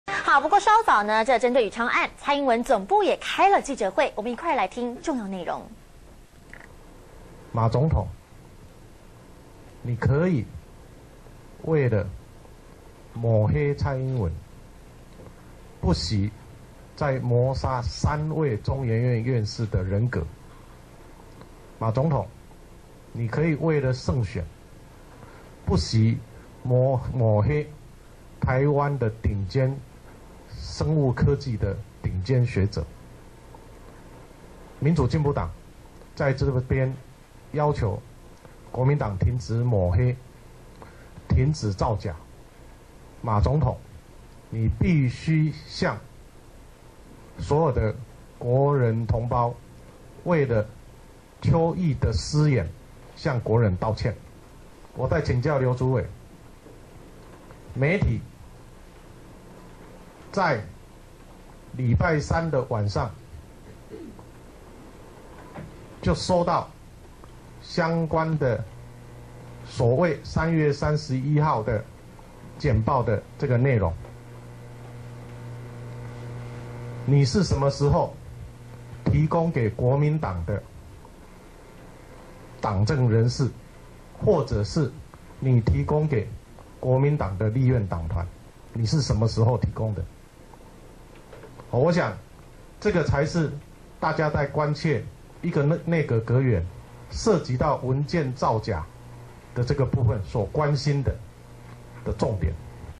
辯論會前止血 民黨回擊談「宇昌」批馬
中天新聞》民進黨總統候選人蔡英文陷入宇昌自肥爭議，辯論會前突然召開記者會批馬政治追殺，民進黨17日一早也加碼召開宇昌案記者會痛批總統馬英九。